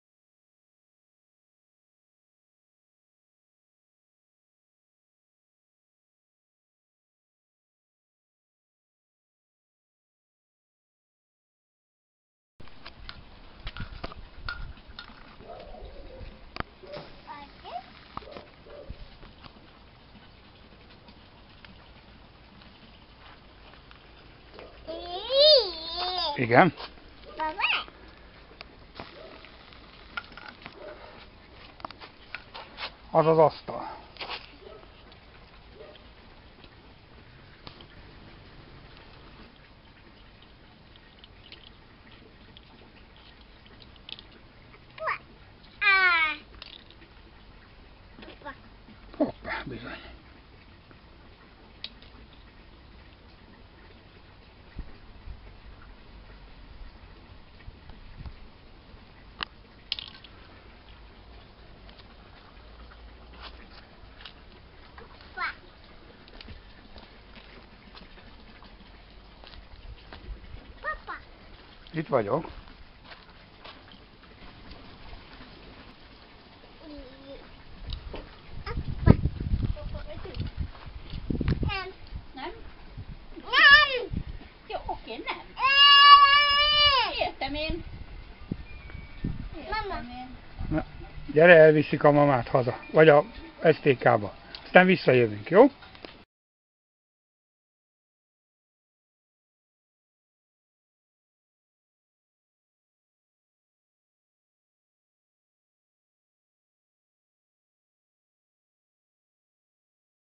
R.K.V.t.
Az én kerti vasutam. Pontosan Caprera-alsó peronján érzi magát  leg jobban az unokám.  Itt nagyon szeret babrálni.